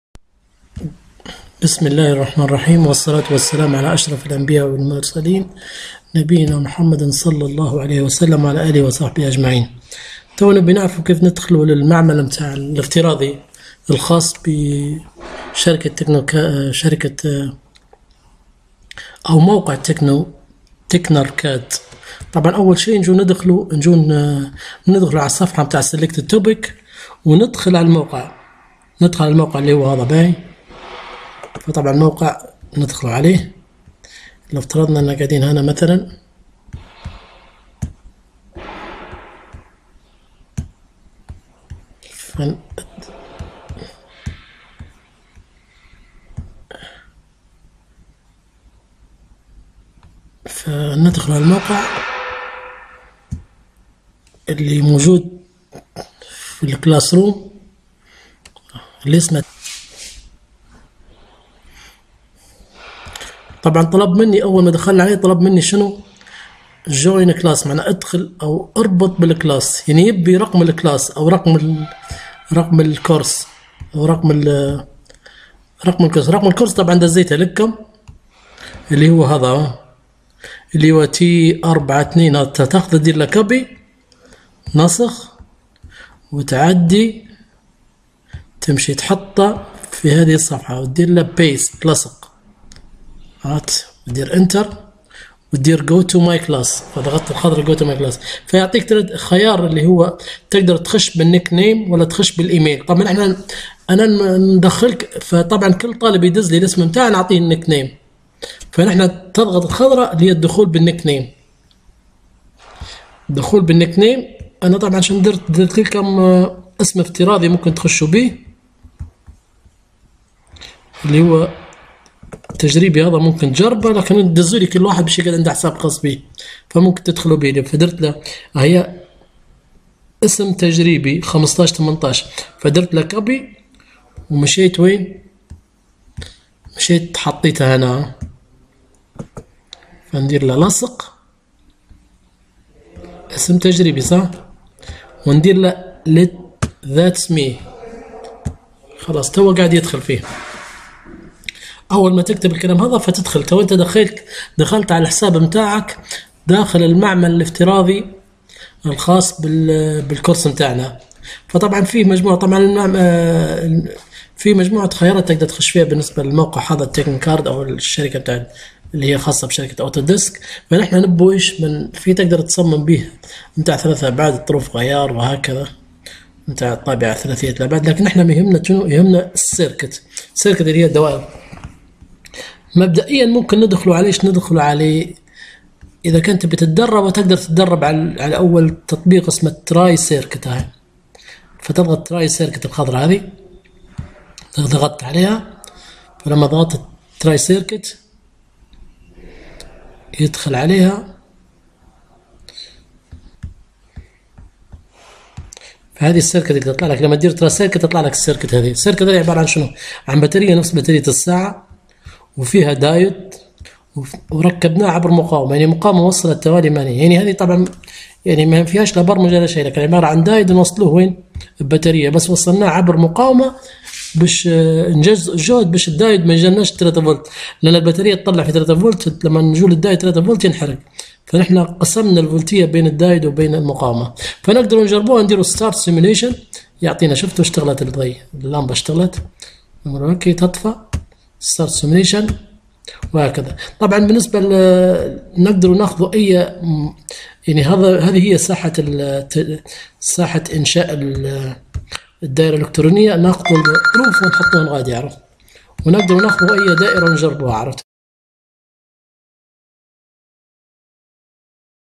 blinky block مادة موضوع مختار Selected Topicهذا الفيديو لمعرفة طريقة انشاء حساب بموقع المحاكاه
القسم :  تقنية الطاقة المتجددة
الفصل :السادس
رمز المادة :SRE346
اسم المحاضر : أ. غيث ارويس